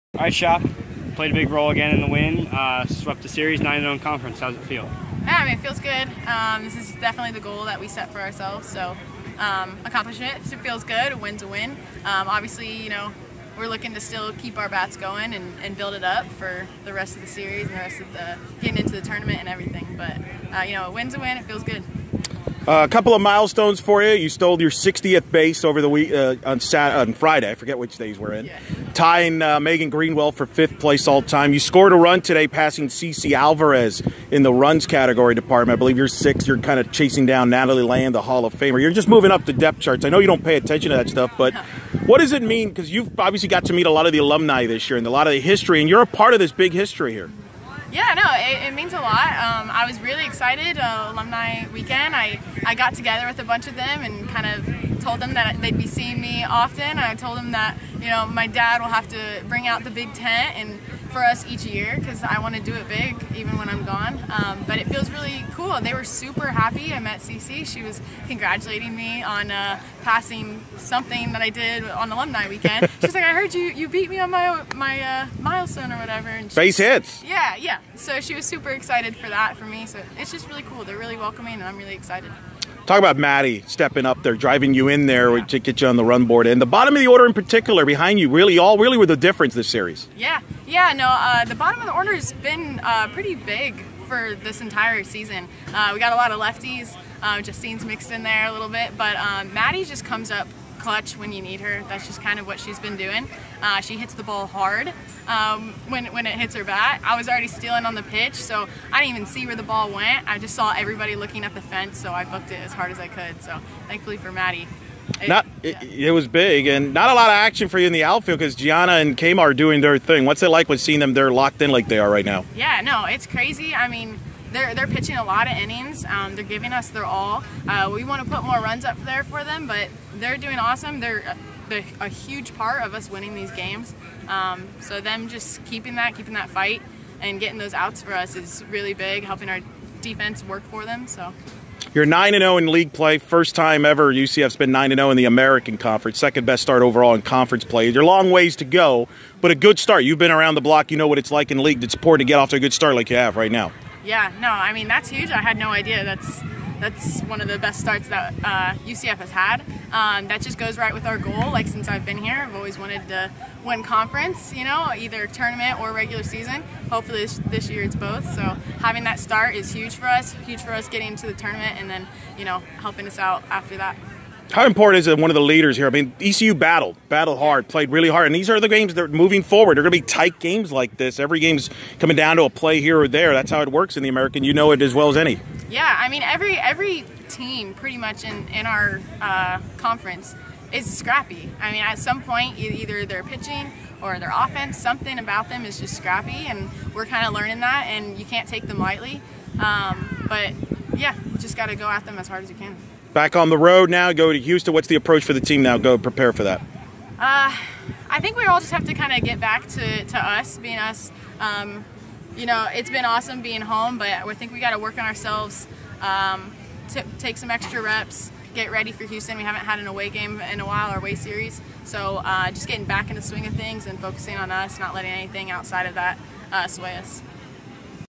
Denali Schappacher postgame ECU
OF Denali Schappacher talks with Black and Gold Banneret following the Knights 2-1 win over ECU